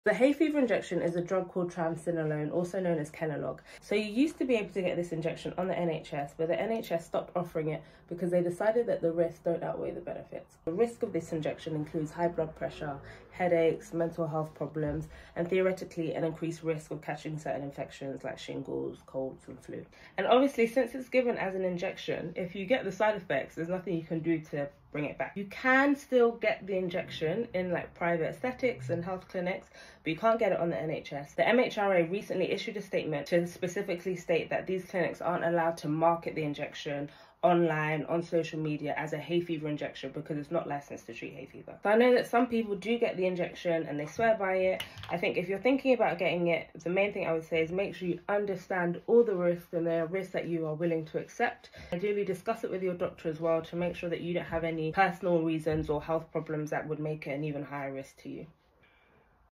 Should You Get the Hay Fever Injection?
Hay fever injections can be an effective treatment option for individuals suffering from severe hay fever symptoms or those seeking long-term relief. However, it is crucial to weigh the benefits against the potential drawbacks and consider individual circumstances and preferences. Consulting with a healthcare professional specialized in allergies can help determine whether hay fever injections are the right choice for you. They can assess the severity of your symptoms, discuss alternative treatments, and guide you in making an informed decision about managing your hay fever effectively.